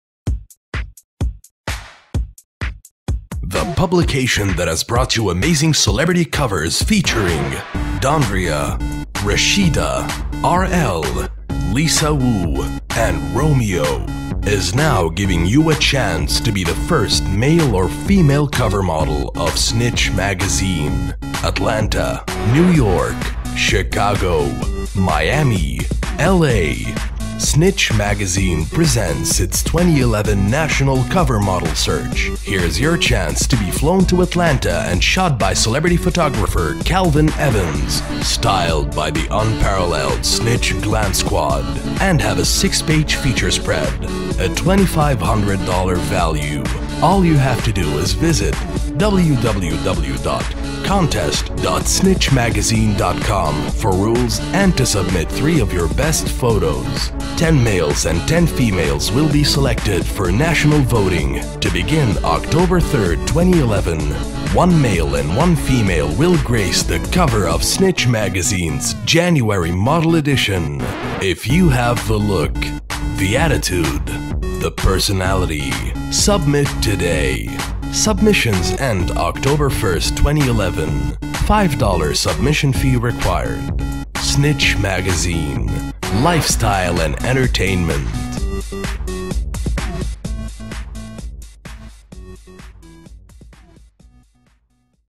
SNYTCH Magazine | 2011 NATIONAL COVER MODEL SEARCH!
Snytch Magazine Presents its 1st Annual National Cover Model Search! 1 Male & 1 Female will be selected to be featured on the January 2012 "Models in Fashion" Issue! for more details!

Voiced By: Elie M. Iskandar
Written By: Snytch Magazine